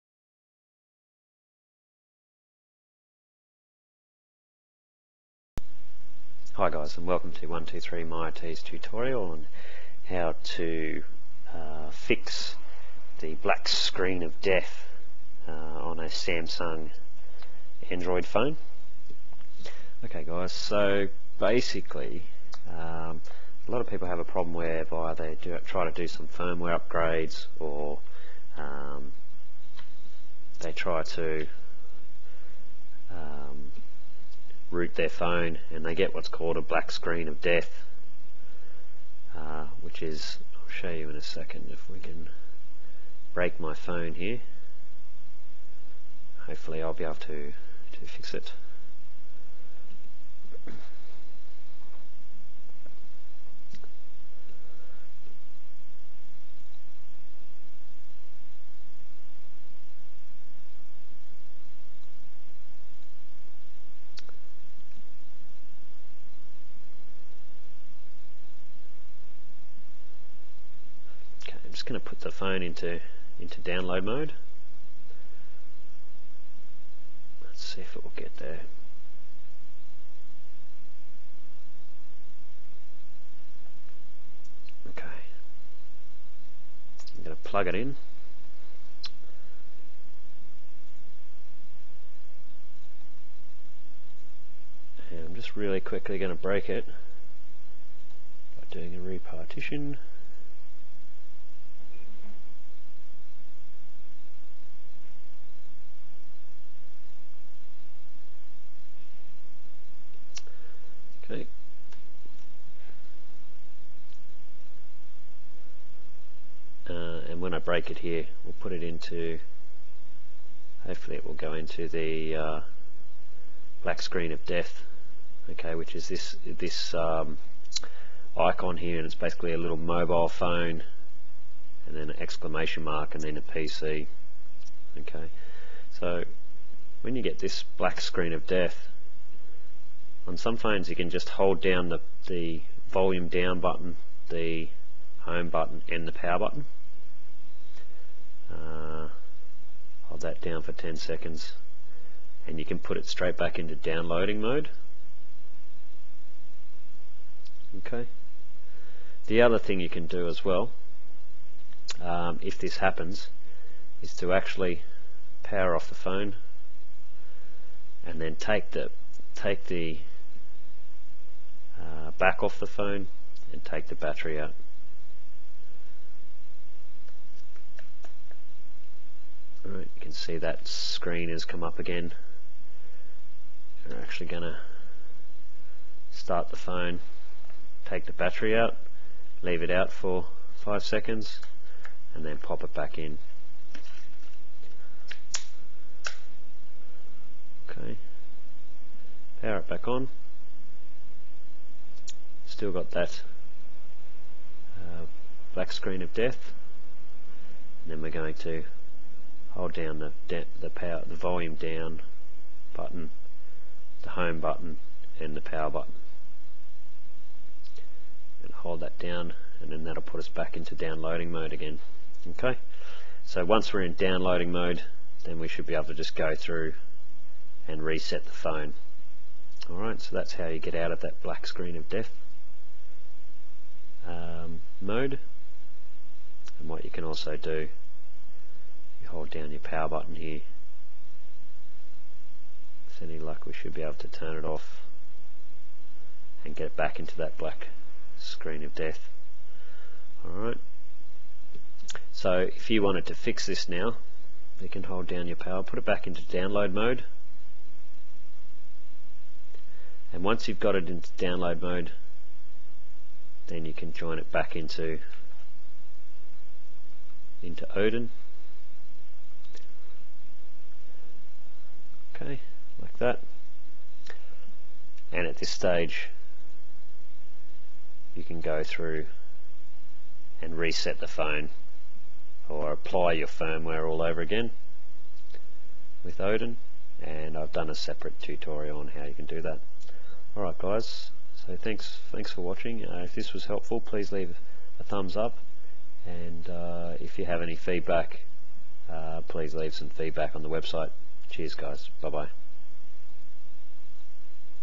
How to get a Samsung Galaxy S phone out of black screen of death.
○○○○○○ Support while shopping at Amazon ○○○○○○

► Amazon US

This video will show you how to get a Samsung Galaxy S phone out of black screen of death.